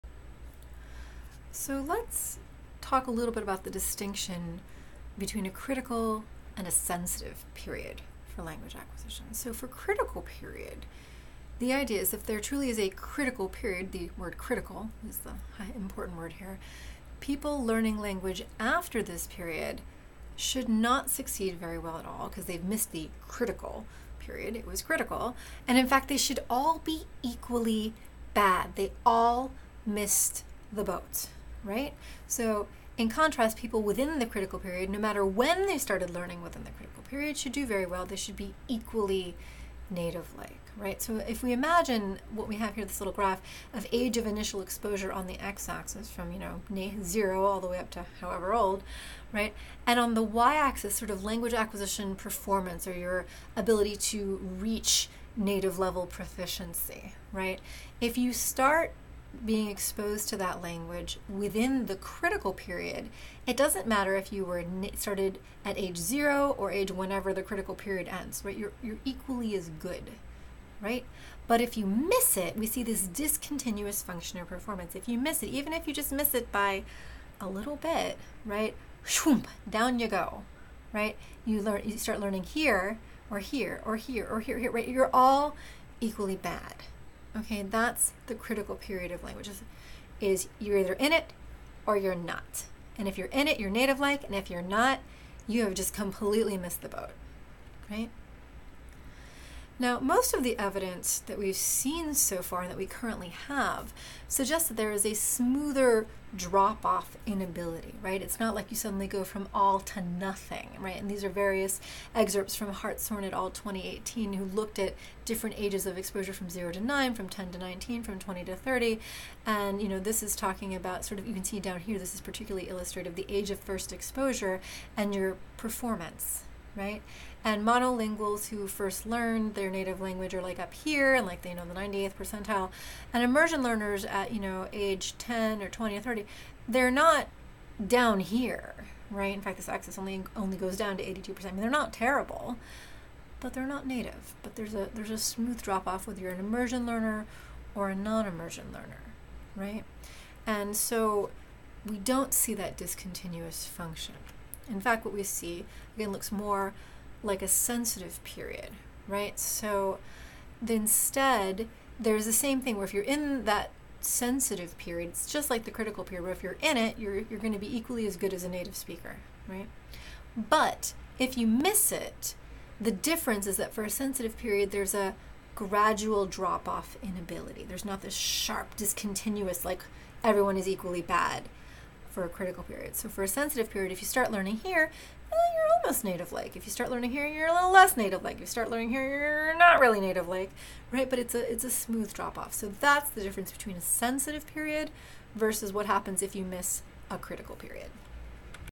AcqOfLang1: A critical vs. a sensitive period for language acquisition
Acquisition of Language 1: The difference between a critical period and a sensitive period for language acquisition